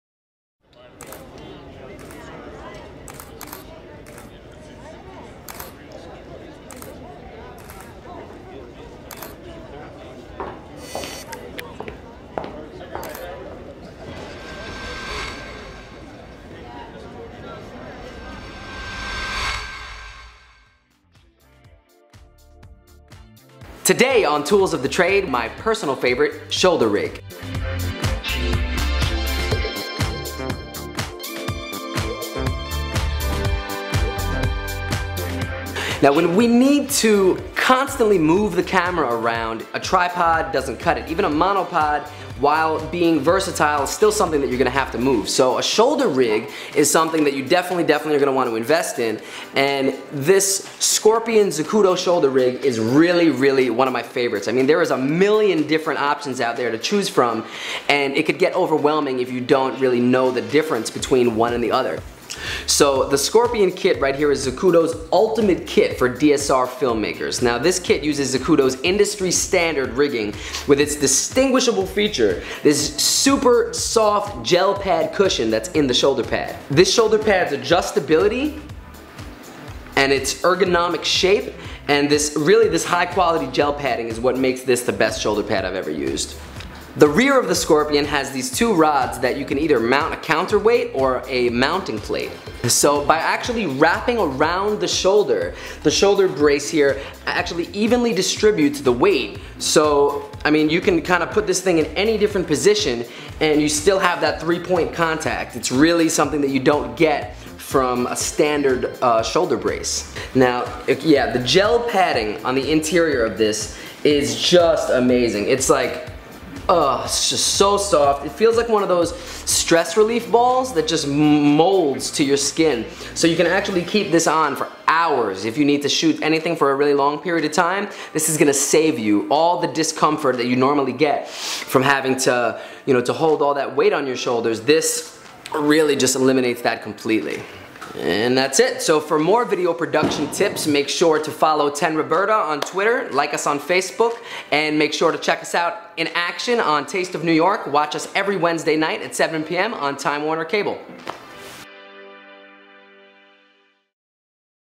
Zacuto Scorpion Rig - Tools of the Trade - Aton Maiti
The Scorpion rig is the ultimate solution to DSLR shoulder mounted shooting with an EVF. At the center of the Scorpion is our highly adjustable DSLR Baseplate, which allows you to mount any DSLR to the rig and mount your rig onto a tripod. The baseplate attaches to your tripod using the standard tripod plate screws; (2) 1/4" 20 and (1) 3/8" 16. You can also easily quick release your camera from the rig by turning the red knob on the side of the baseplate and pulling up on your camera. This unique feature allows you to quickly switch between using the camera on its own or as part of your rig.